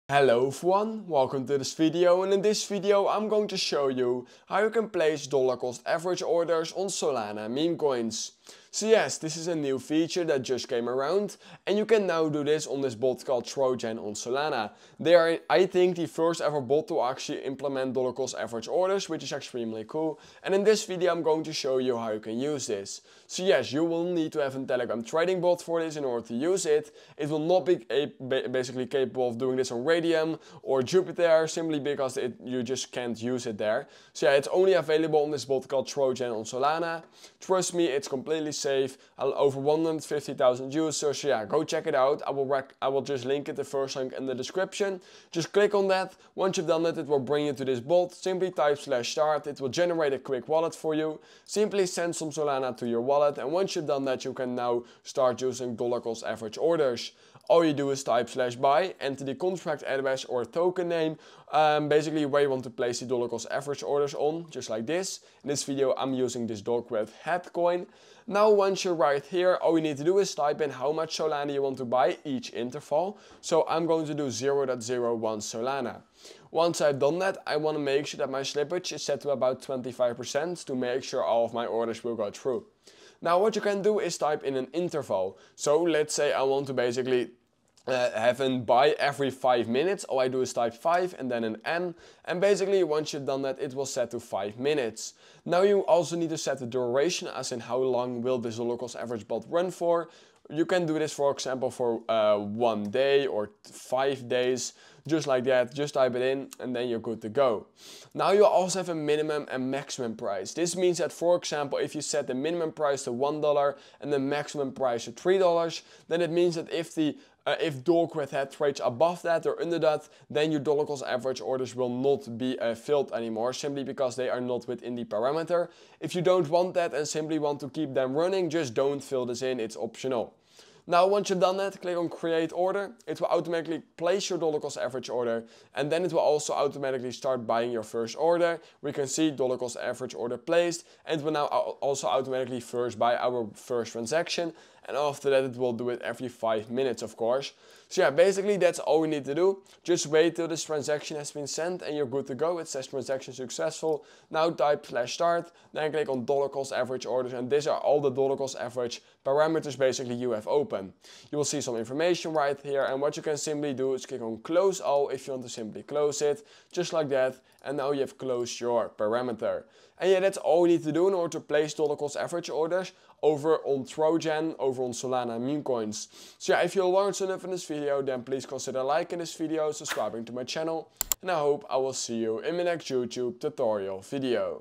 How To Place DCA Orders On Solana Meme Coins
How To Place DCA Orders On Solana Meme Coins - Full Tutorial

👉 In this video we are going to show you how you place DCA orders on Solana meme coins.